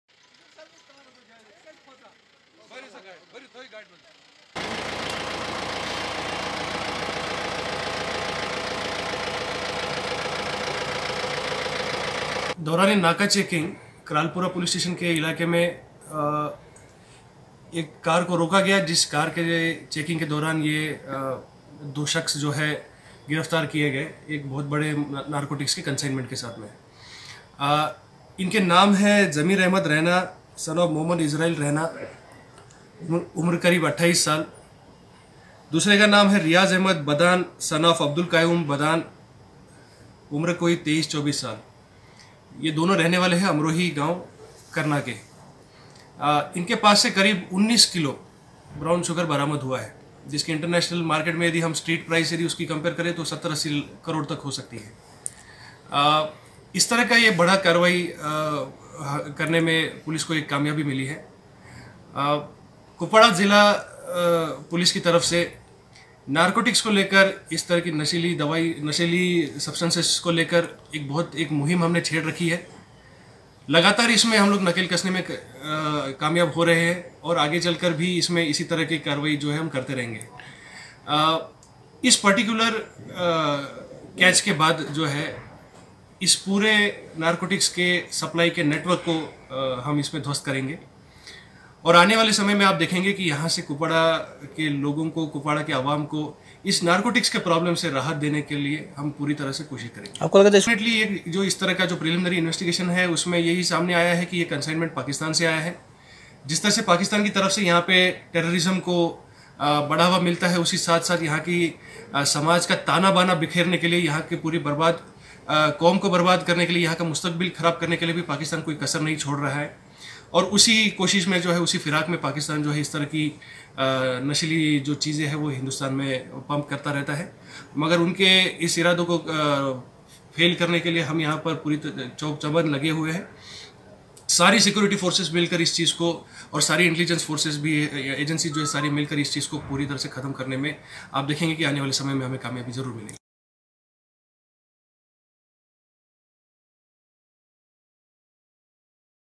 Brown sugar valuing Rs 80 Cr smuggled from Pak seized , 2 accused arrested
| Excelsior News |

Kupwara: Police on Thursday recovered contraband substance, brown sugar, worth Rs 70 Crore worth from Kralpora area of Kupwara district and detained two accused.
The two accused belonging to Karnah were arrested from Kralpora area of the district
Police said that they have cracked a big consignment of Pakistani Narcotics transported through border.
SSP Kupwara, Shririam Ambarkar said that, peliminary investigation into the case has revealed that, the consignments was transported from Pakistan.